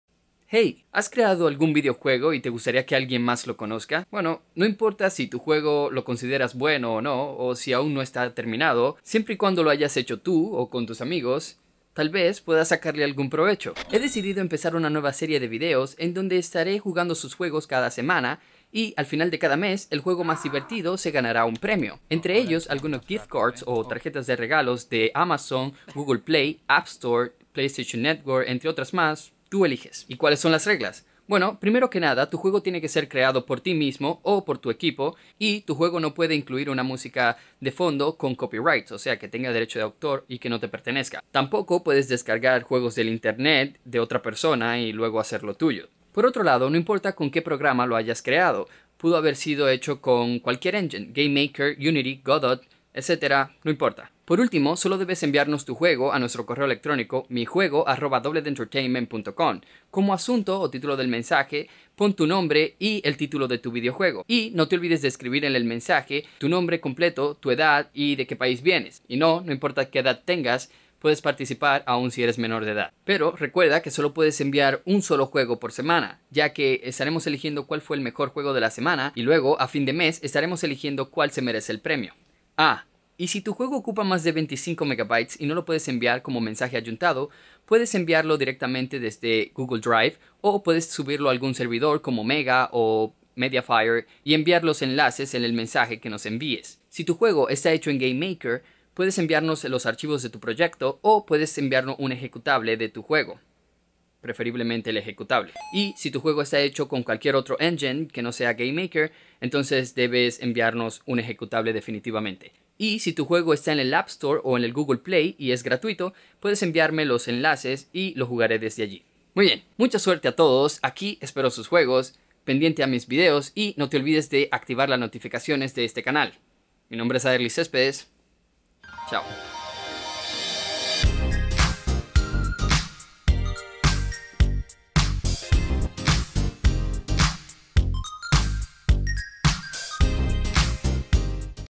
Como hacer dinero con tus juegos 🎮 - CONCURSO 2018
REGLAS: (LEER)
1- Tu juego tiene que ser creado por ti o tu equipo

3- No puedes usar el juego de otra gente y fingir que es tuyo

4- Tienes que enviar un .exe (ejecutable) o .zip de tu juego. (Si usas GameMaker también puedes enviar el proyecto)

5- Solo puedes enviar un juego por semana.

PREMIOS:
Gift Cards (Tarjetas de Regalo) de:
- Amazon
- Google Play Store
- App Store & iTunes
- eBay

PREGUNTAS FRECUENTES:
• Puedo participar si soy menor de Edad?
 - Sí

• Si me juego está en el App Store, Play Store o cualquier otro Store o pagina, puedo participar?
 - Sí, siempre y cuando sea gratuito.

• Mi juego lo hice hace tiempo, puedo participar?
 - Sí, no importa cuando lo hayas hecho.

• Participé con este juego en uno de tus concursos, puedo participar con el mismo juego?
 - Sí, este es un nuevo concurso y puedes participar con el mismo juego.

• Puedo elegir mi premio, es decir que Gift Card prefiero?
 - Sí, puedes elegir cualquier Gift Card que esté en la lista.

• Mi juego ocupa mucho espacio y no puedo enviarlo por correo, por donde lo envío?
 - Puedes usar cualquier servidor gratuito, como: Mediafire, Mega, etc. Y envíanos el enlace con las instrucciones ya mencionadas.

• Puedo utilizar imágenes, música o sonidos de usos libres?

• Cuando expira este concurso?
 - Por ahora no hay fecha de expiración, la idea es hacer un concurso cada mes con los juegos que me envíen durante las semanas de cada mes.

• Entonces si pierdo puedo volver a participar en el próximo mes?
 - Sí, siempre y cuando sea un juego distinto; de hecho puedes enviar un total de 4 juegos diferentes al mes, cada semana puedes enviar 1 juego (pero no mas de uno por semana)

• Si gano este mes, puedo participar con un juego diferente el próximo mes?
 - Sí, puedes participar todos los meses, siempre y cuando sea un juego distinto al anterior.